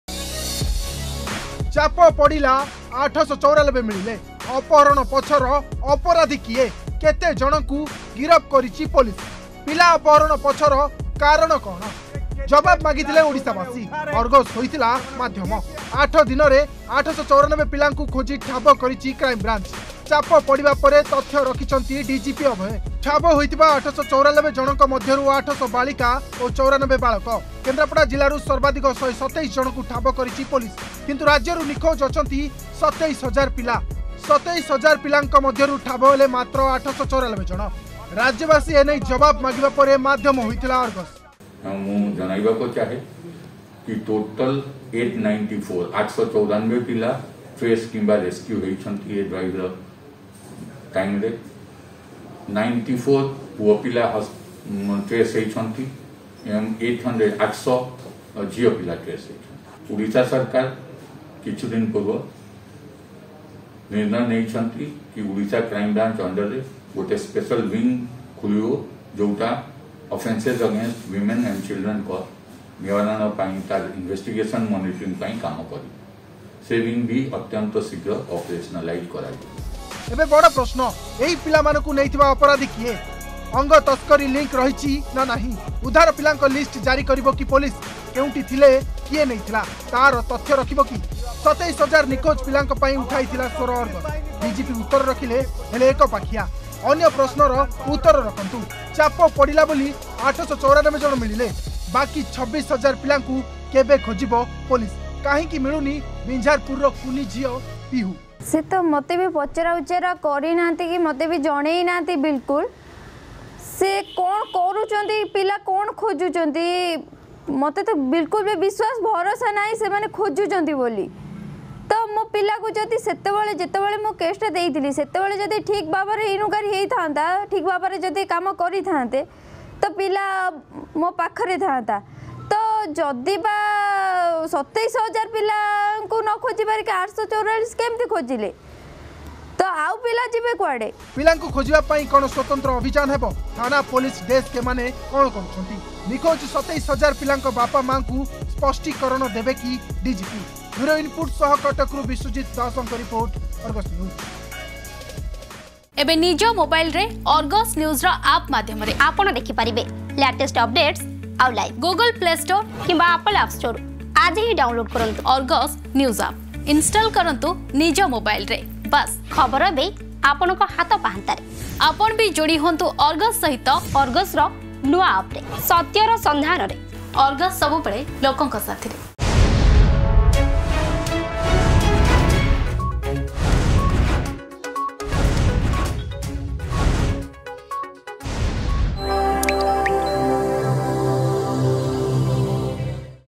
ଜବାବ ମାଗିଥିଲେ ଓଡ଼ିଶାବାସୀ
ଜବାବ ମାଗିଥିଲେ ଓଡ଼ିଶାବାସୀ, ଅର୍ଗସ ହୋଇଥିଲା ମାଧ୍ୟମ । 8 ଦିନରେ 894 ପିଲାଙ୍କୁ ଖୋଜିଛି ଓ ଠାବ କରିଛି କ୍ରାଇମବ୍ରାଞ୍ଚ । ଚାପ ପଡିବା ପରେ ତଥ୍ୟ ରଖିଛନ୍ତି ଡିଜିପି ଅଭୟ । ଠାବ ହୋଇଥିବା 894 ଜଣଙ୍କ ମଧ୍ୟରୁ 800 ବାଳିକା ଓ 94 ବାଳକ । କେନ୍ଦ୍ରାପଡା ଜିଲ୍ଲାରୁ ସର୍ବାଧିକ 127 ଜଣଙ୍କୁ ଠାବ କରିଛି ପୋଲିସ । କିନ୍ତୁ ରାଜ୍ୟରୁ ନିଖୋଜ ଅଛନ୍ତି 27 ହଜାର ପିଲା । 27 ହଜାର ପିଲାରୁ ଠାବ ହେଲେ ମାତ୍ର 894 । ରାଜ୍ୟବାସୀ ଜବାବ ମାଗିବା ପରେ ମାଧ୍ୟମ ହୋଇଥିଲା ଅର୍ଗସ୍.
ଅଧିକ ଖବର ପାଇଁ ଡ଼ାଉନଲୋଡ କରନ୍ତୁ “Argus News” ଆପ୍